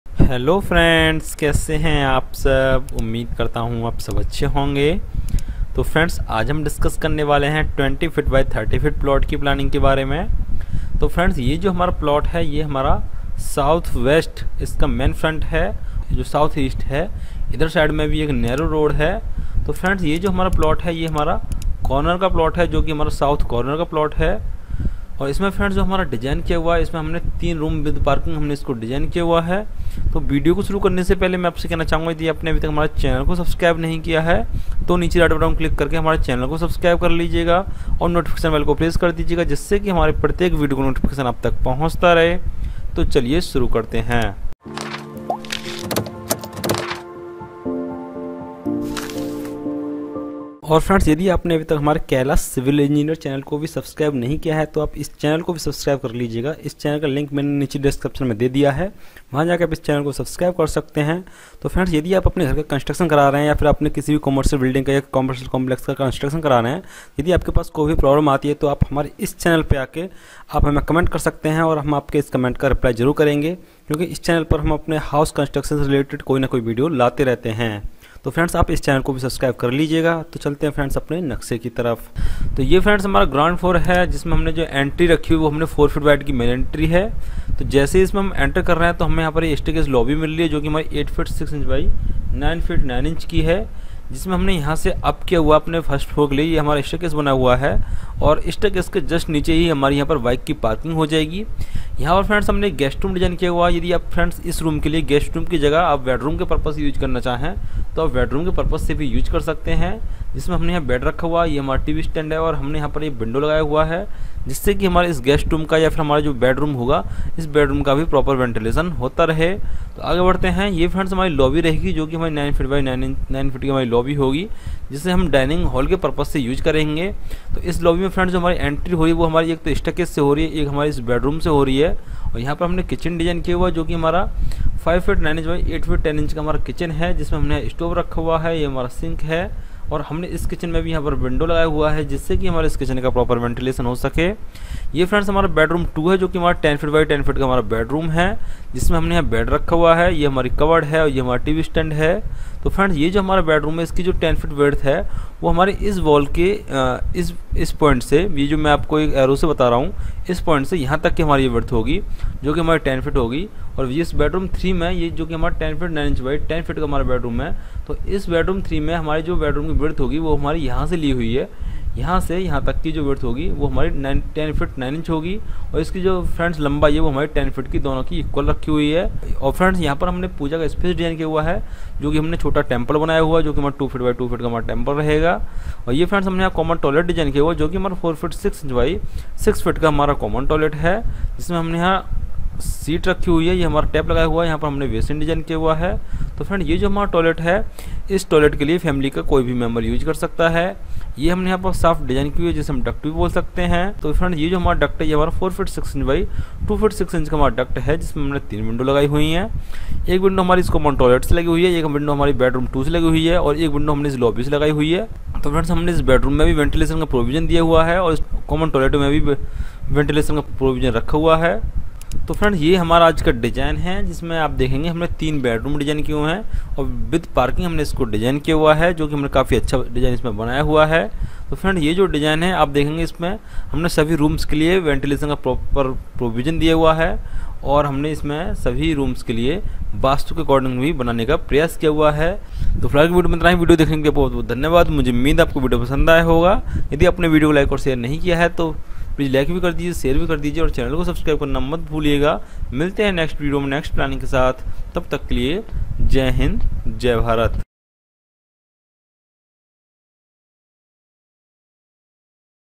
20'-0"x30'-0" House Map | 3 Bedroom House With Parking | Gopal Architecture
Hii
I am Gopal.
Friends aaj ki video mai me aapse discuss krunga 20'x30' squre feet House ki Planinng k bare mai.

House Plan, Home Plan, 3D Interior, 3D elevation, House Design, Gopal Architecture, 3D ideas, Floor Plans, Architecture, Interior, Structure, Construction, Software Tutorial etc. And More Knowledge

Thanks.